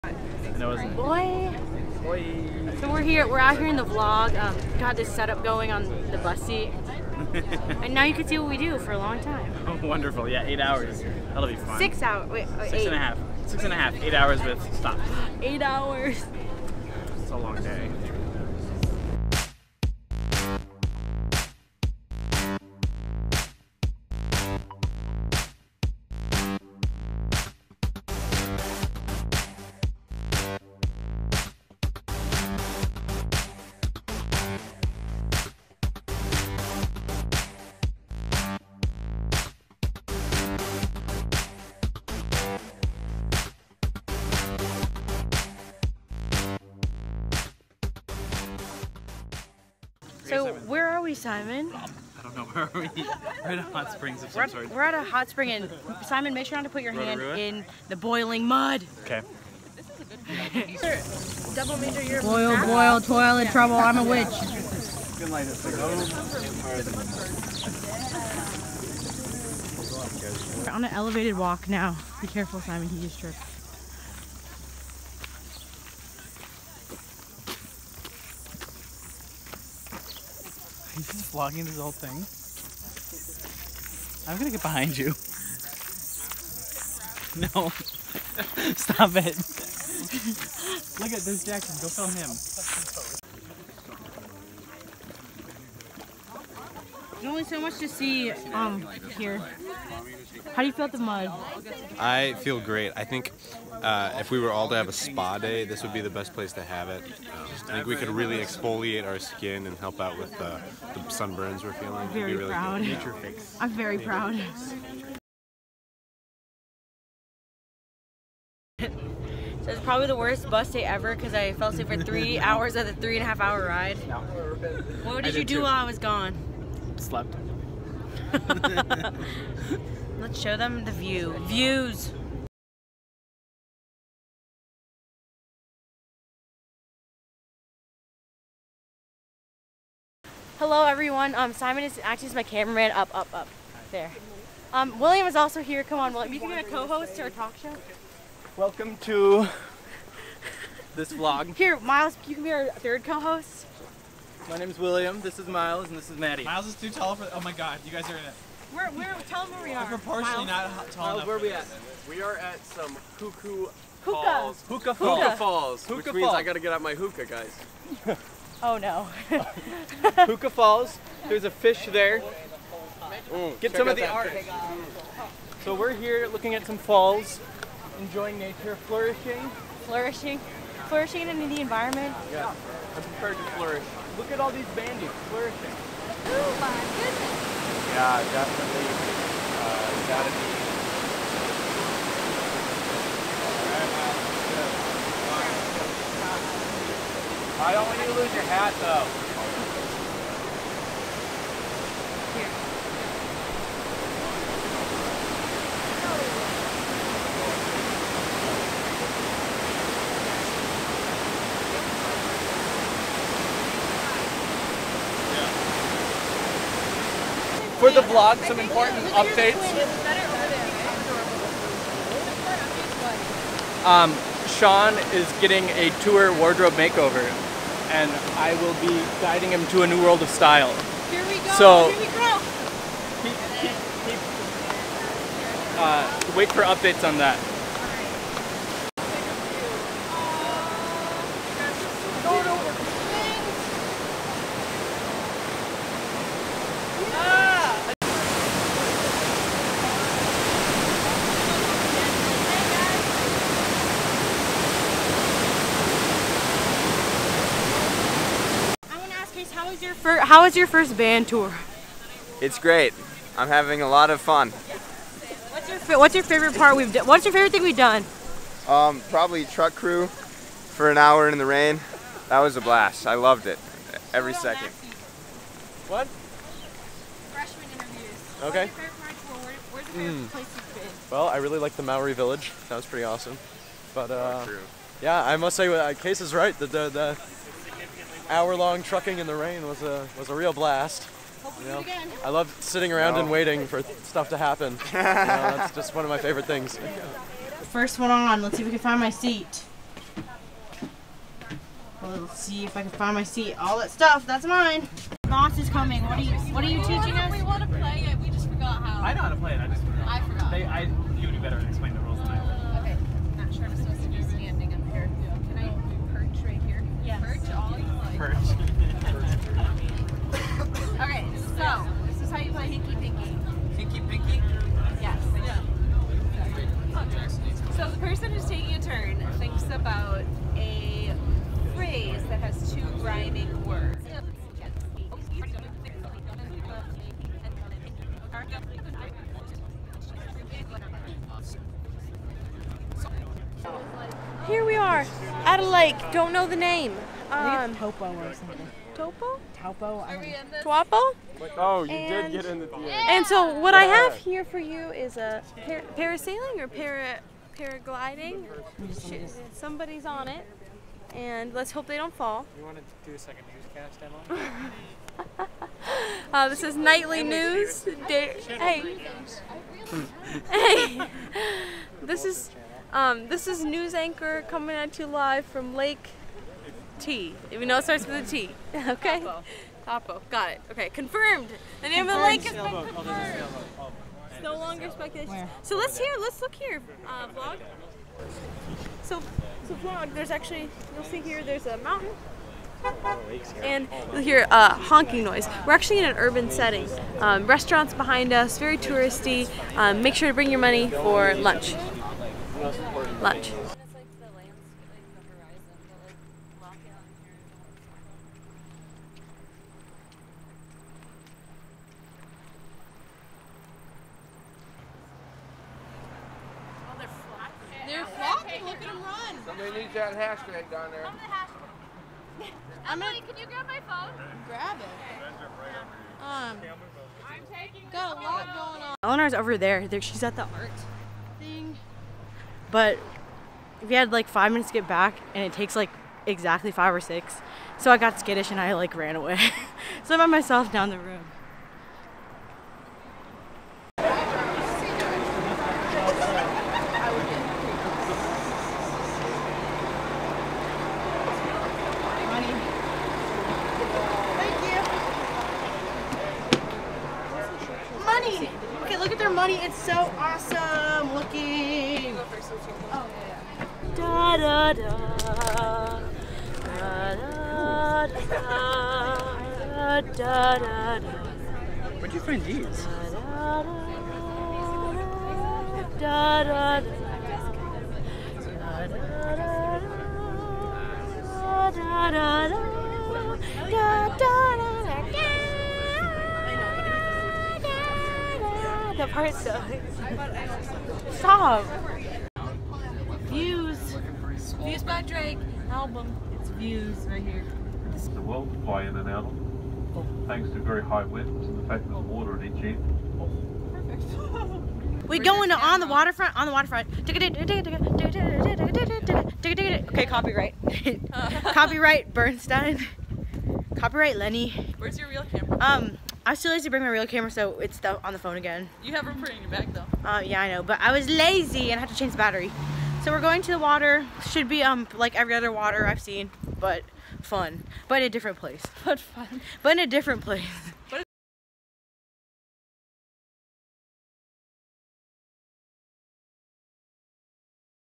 Band Tour 2018 Day 6: NEW ZEALAND BUS DAY
We have a looooong drive from Rotorua to Wellington where we visit a boiling mud pit, Huka Falls, and Lake Taupo!